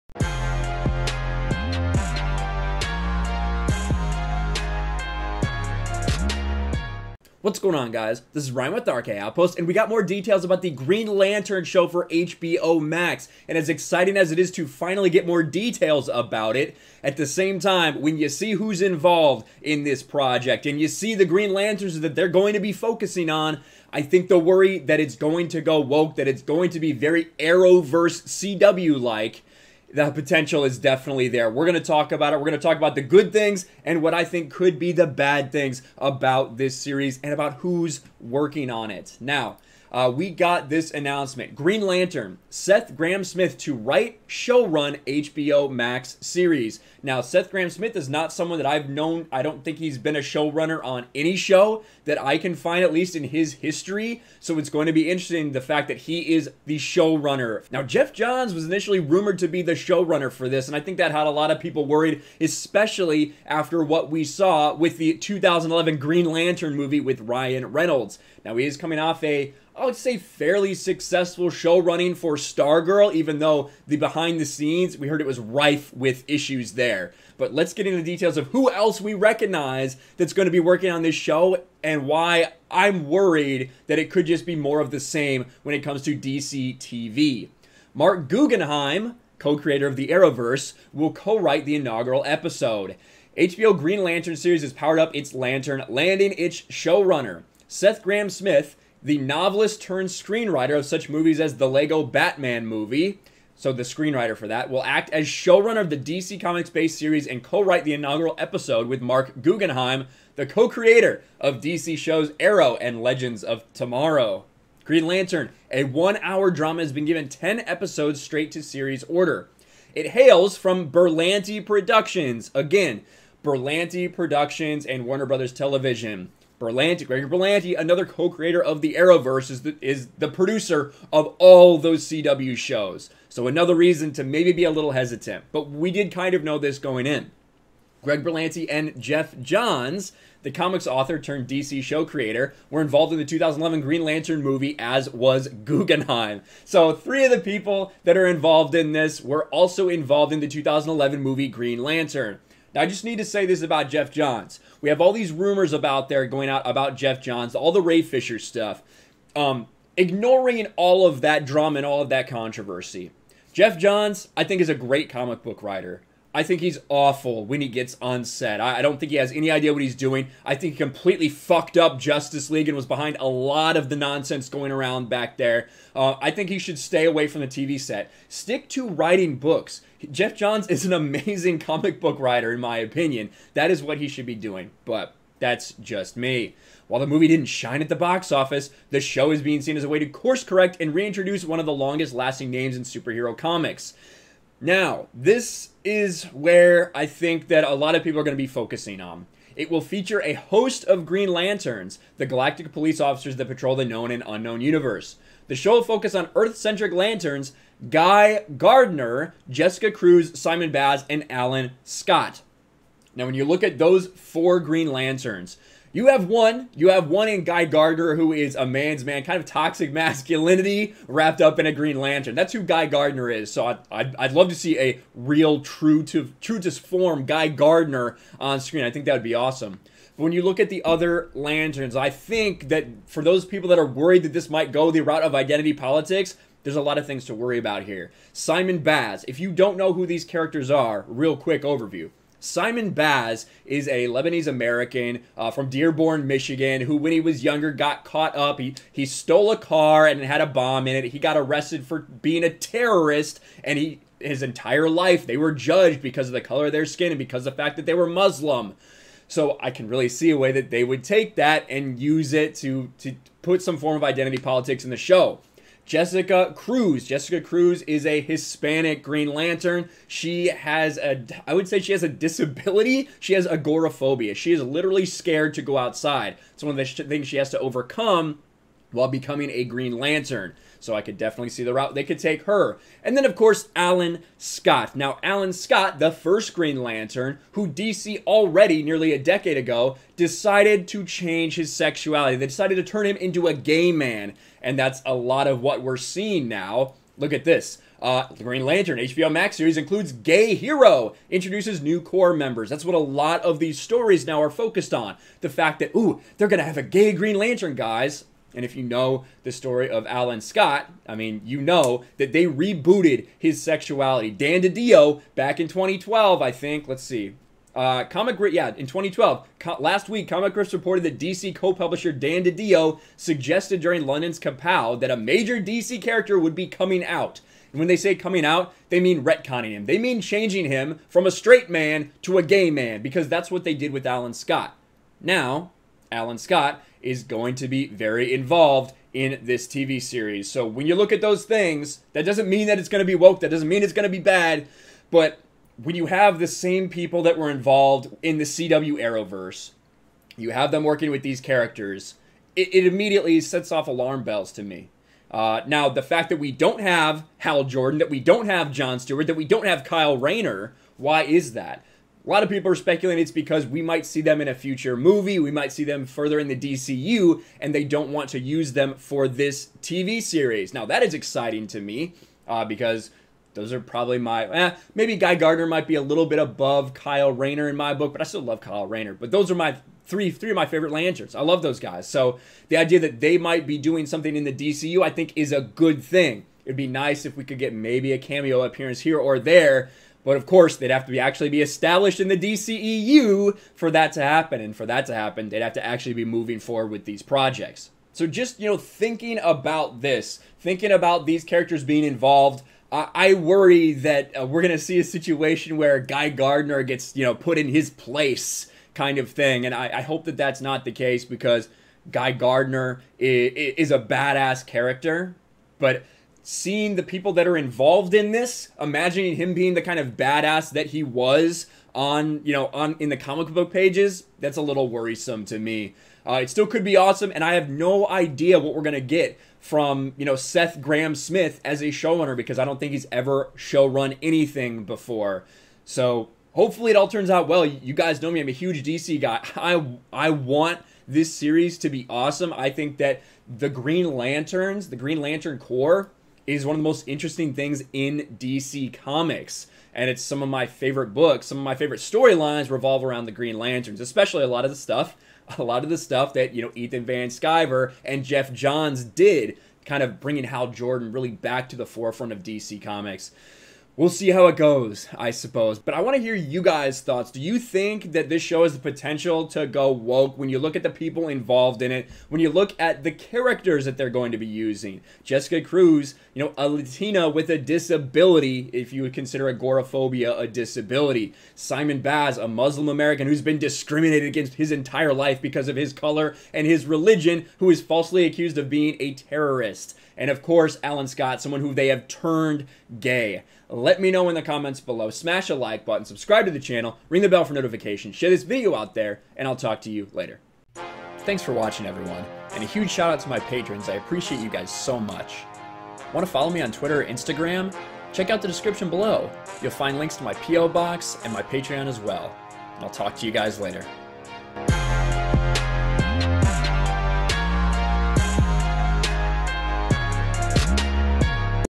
Green Lantern HBO Max Series Details | Will It Go Full CW Woke?
We got the full details about the Green Lantern series for HBO Max, and while the details are exciting, it definitely has the potential to go full CW Arrowverse and be woke.

Want to send me something?  That's awesome!